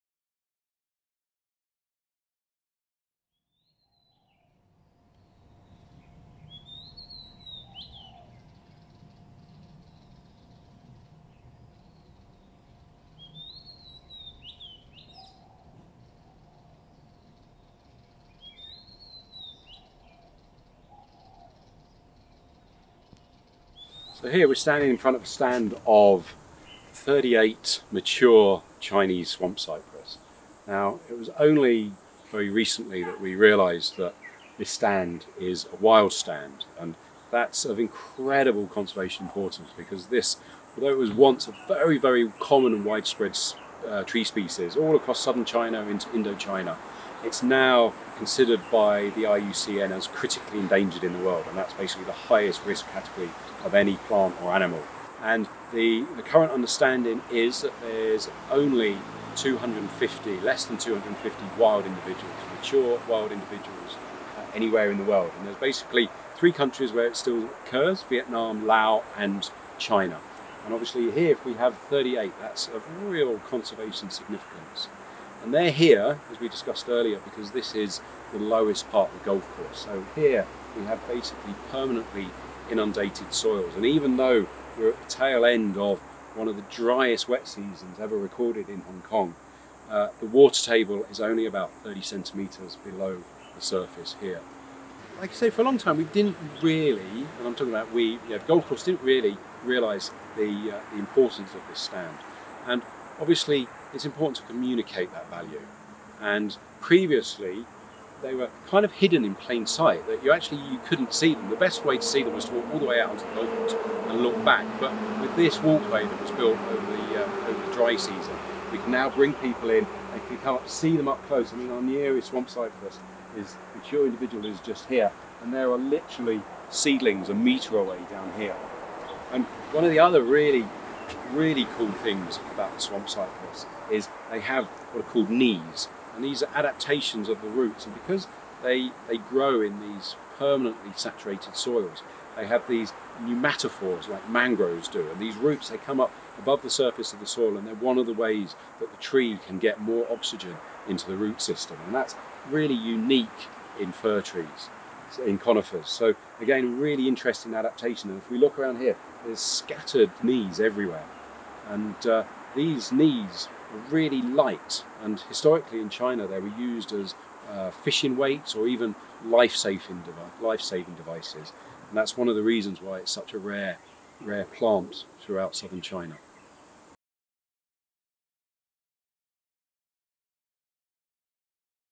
香港哥爾夫球會與環境管理：粉嶺舊球場7號洞 - 下集 || HKGC Ecology Old Course #7 Part 2
球會生態顧問利雅德先生將介紹位於粉嶺舊球場7號洞之稀有中國野生水松群。

Our ecology consultant Paul Leader discusses the incredibly rare and Critically Endangered stand of Chinese Swamp Cypress trees, which can be found on the 7th hole of the Old Course.